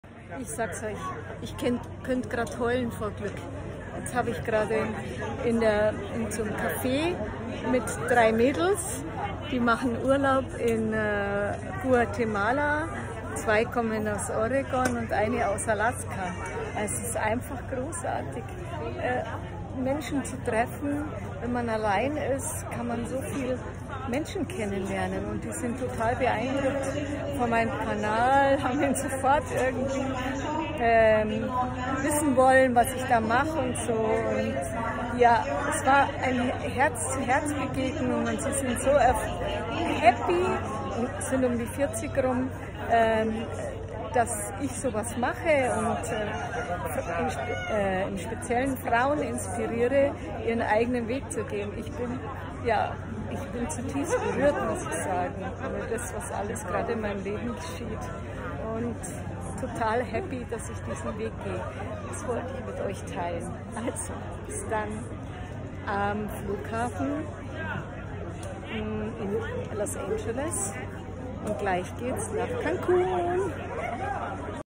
Was passiert, wenn man als Frau alleine durch die Welt reist?
Das kann ich mir gar nicht ausmalen, wie schnell sich neue wundervolle Begegnungen ergeben, einfach spontan aus dem Herzen heraus. Dafür schätze ich mein Leben.

I can't imagine how quickly new wonderful encounters arise, simply spontaneously from the heart. That's what I value my life for.